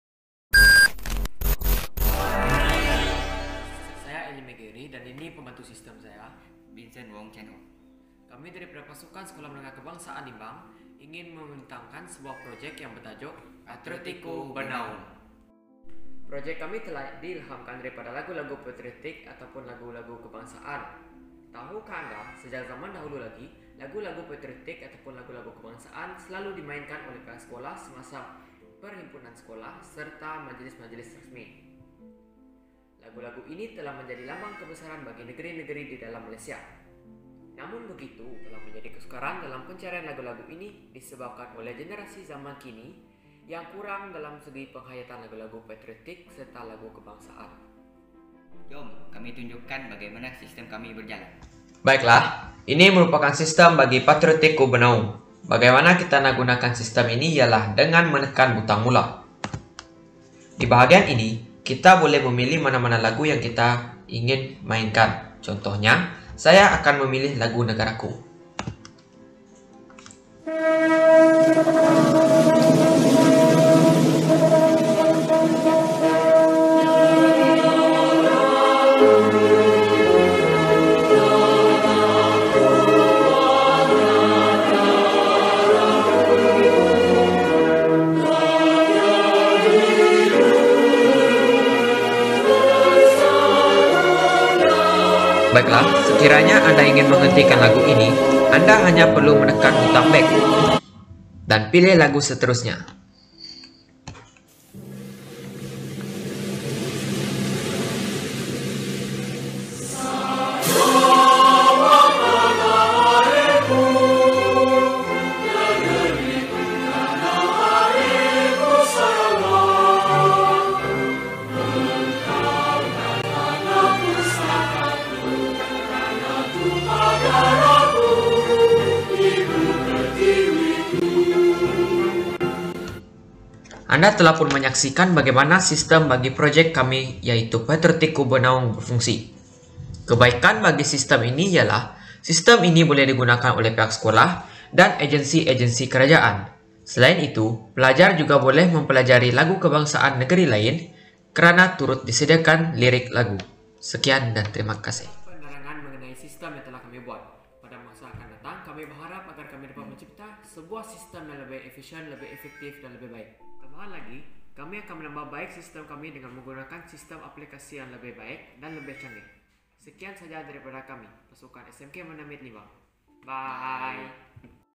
Sistem "Patriotikku Bernaung"
Sistem ini dicipta bagi memudahkan pengurusan dalam nyanyian lagu2 negeri dan kebangsaan di sekolah-sekolah dan badan -badan kerajaan. Sistem ini sebagai satu sumber inspirasi kepada anak muda tentang lagu-lagu patriotik bahkan dapat membantu dalam memupuk semangat cinta akan tanah air.